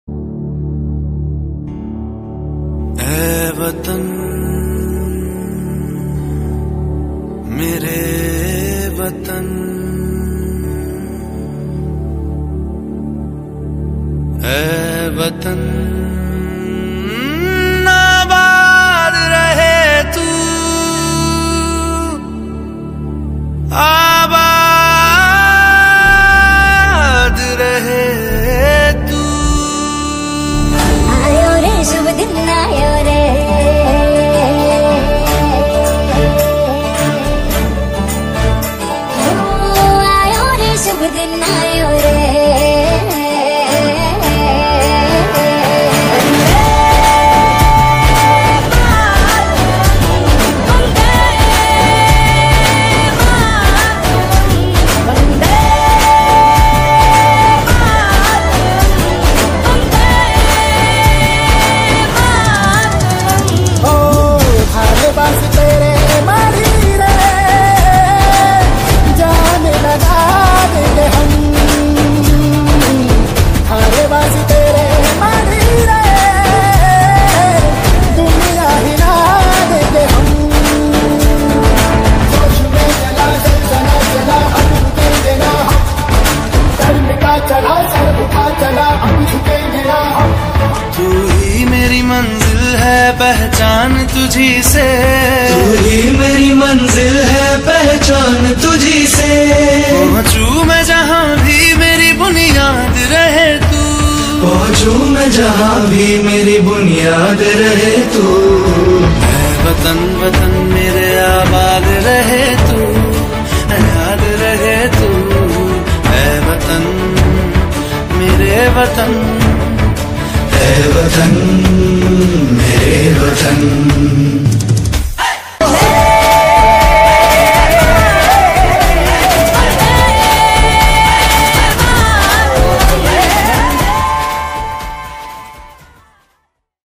Happy Independence Day
The glory of Independence Day comes from the sacrifice of our brave-hearts.
With pride in the hearts the students celebrated the 75th Independence Day in their little ways.
The students of BBIS participated in different activities like kite decoration, Tricolour kites making, Tricolour flowerpot making, Tricolour peacock making, Patriotic song competition, and promising freedom from one bad habit through the activity Let's be truely free.
All the teachers left no stone unturned to make it a fun filled day for all.